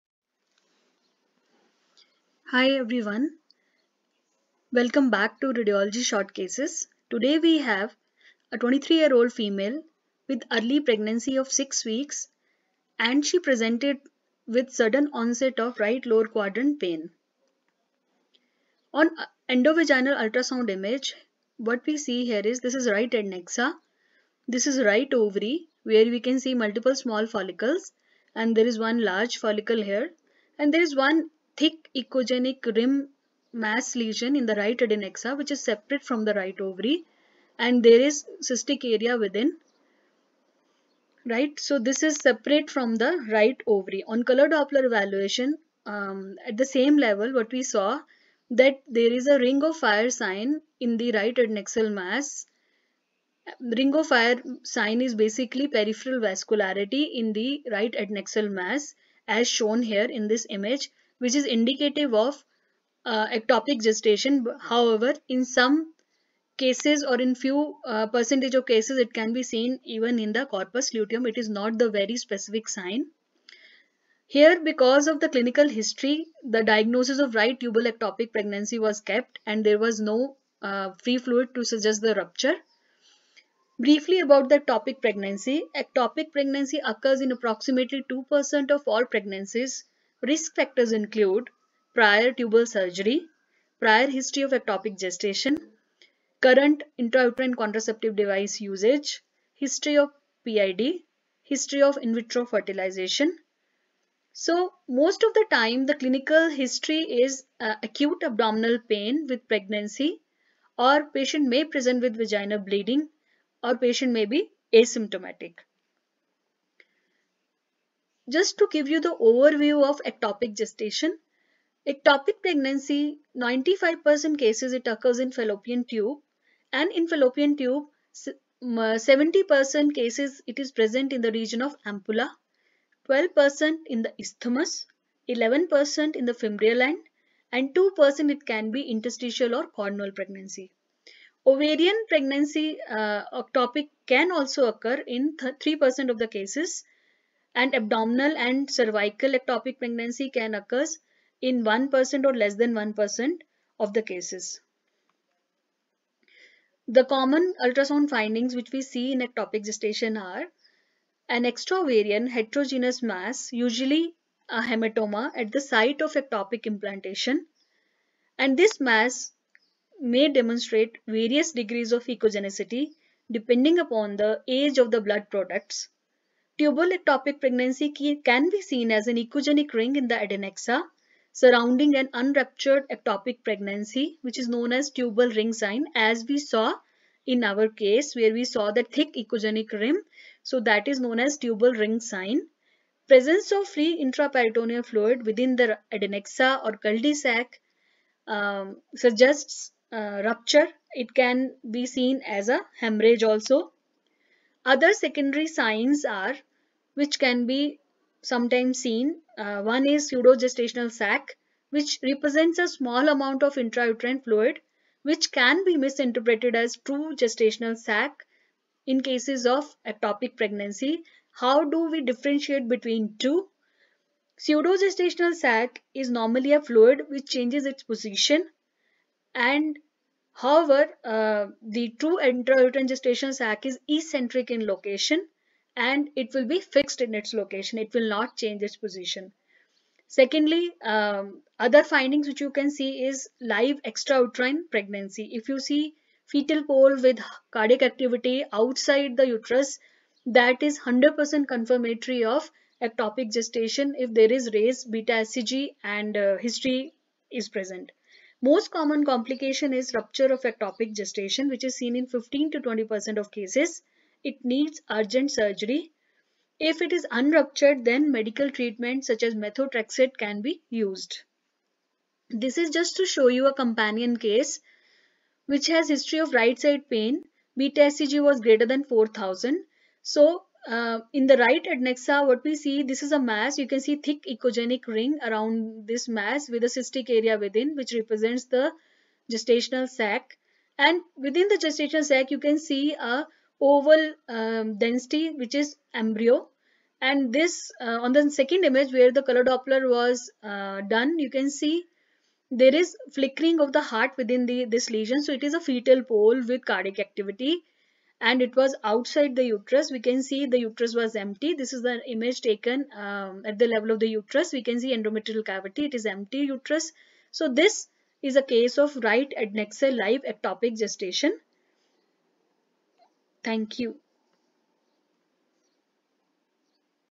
Ectopic Pregnancy|| Radiology Short Cases
Sonographic findings of ectopic gestation explained.